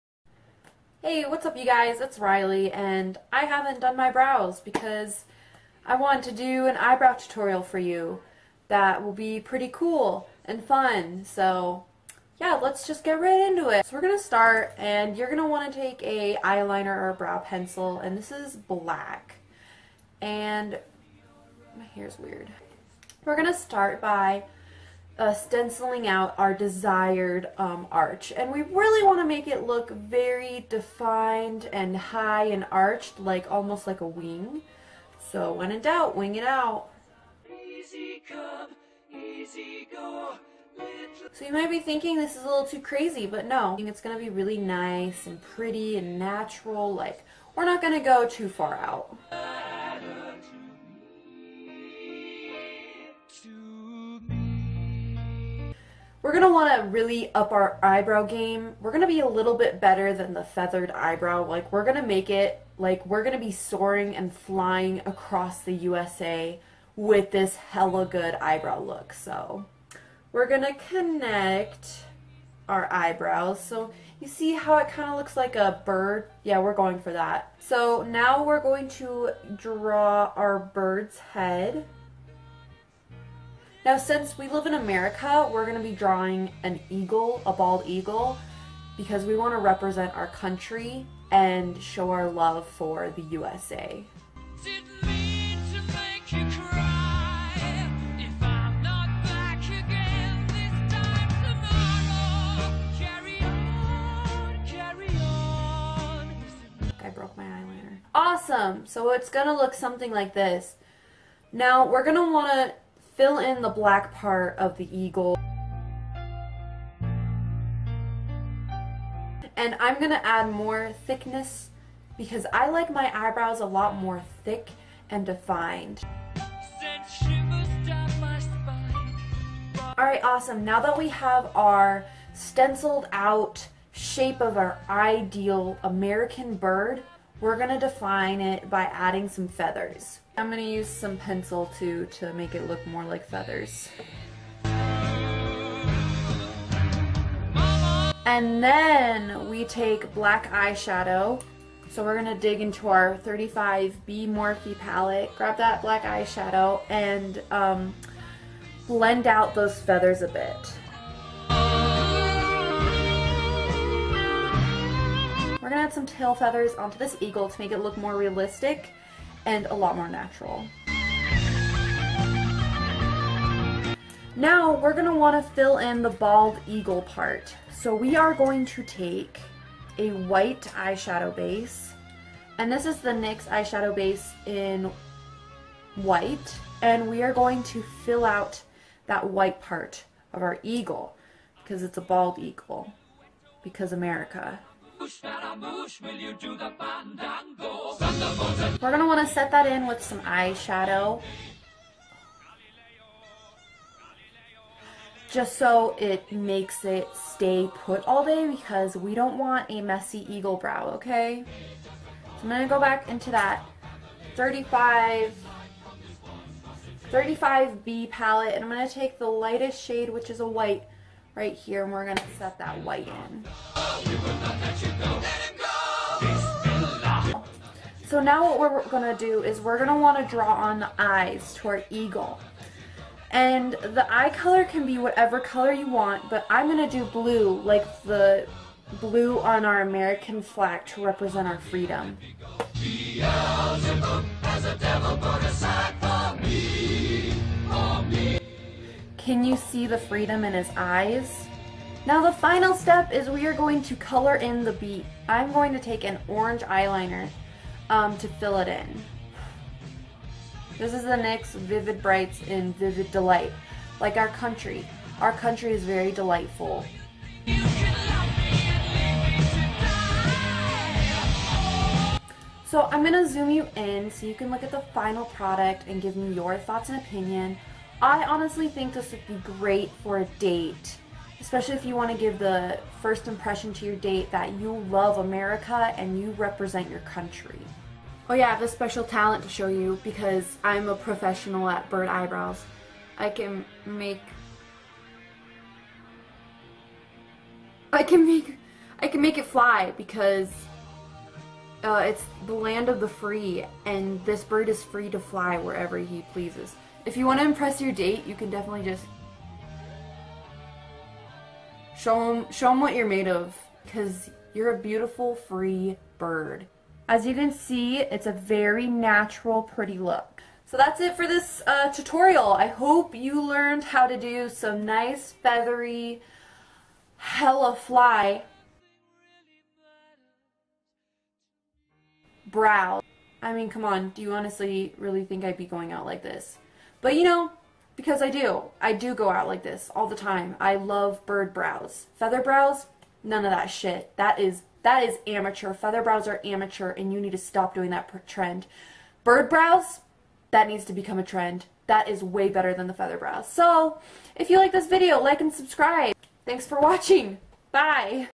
better than your feather brow tutorial
Haha ayyy as you can tell if you didn't already this is a complete joke! I wanted to poke fun at this new feather eyebrow trend by taking it to the next level of just doing a bird brow! Go big or go home amirite? I hope you got good fun out of this video! Also, I'm not saying the feather brow trend is bad. Makeup is art and a way to express yourself! This is just a parody. (Just a p r a n k bruh.)
Also, let's make bird brow a trend please

Social Media!